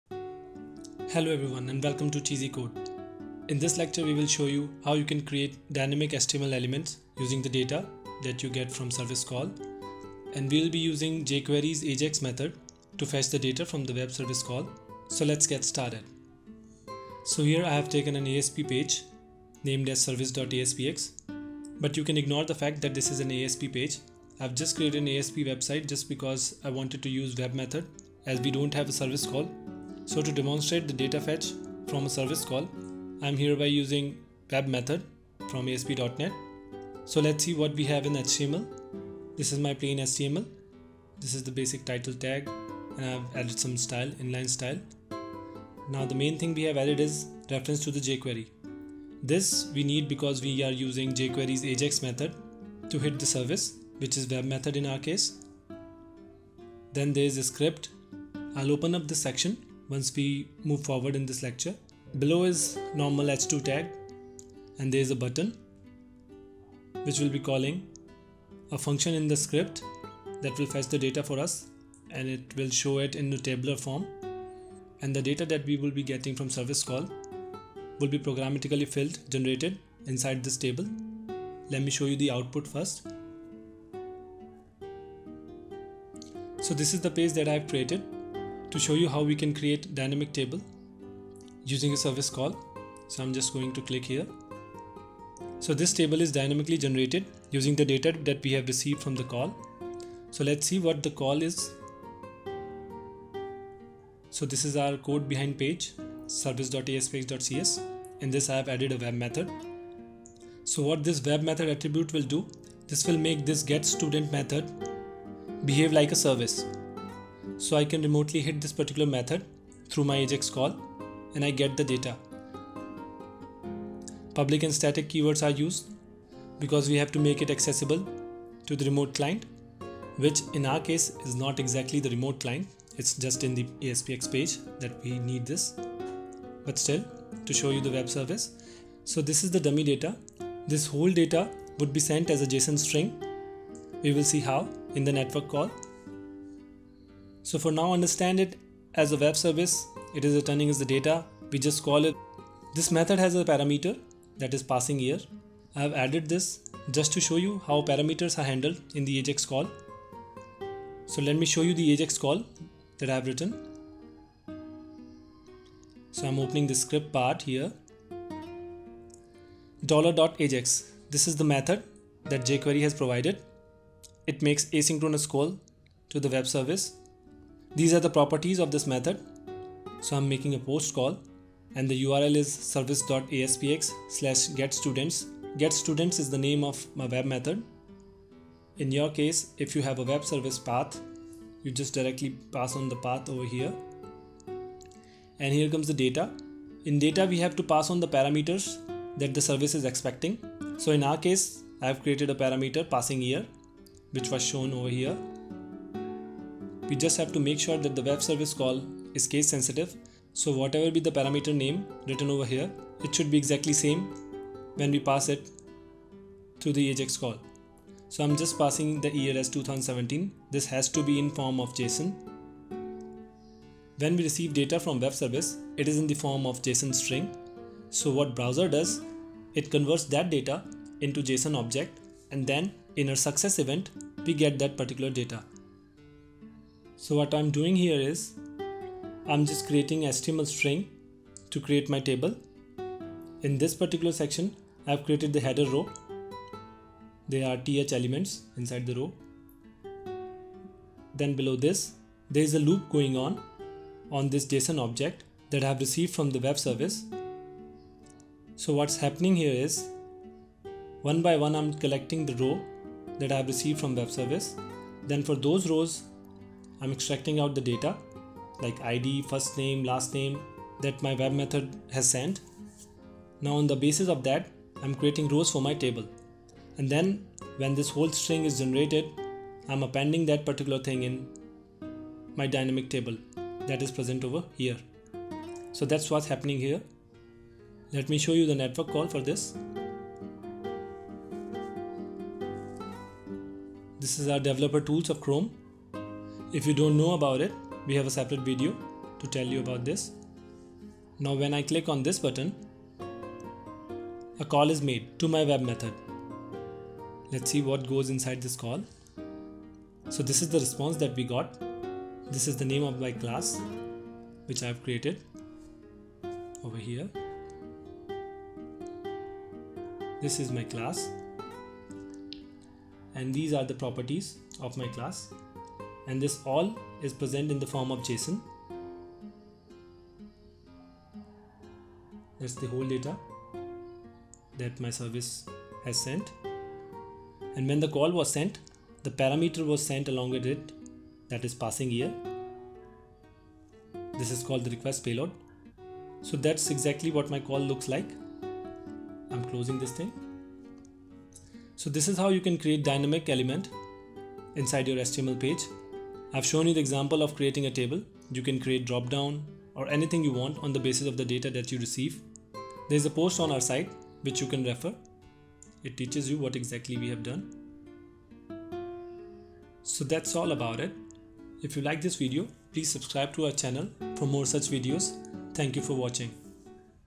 Create Html Table In JavaScript Using WebService Ajax Call
In this video you will learn to create HTML table or other elements using jQuery, AJAX and Asp .NET WebMethod without postback.

What you can expect from this video:
Some basic HTML learning to create structure for table
Jquery $.ajax method and it's parameters
Creation of WebMethod for ajax call
Using JSON object to create dynamic elements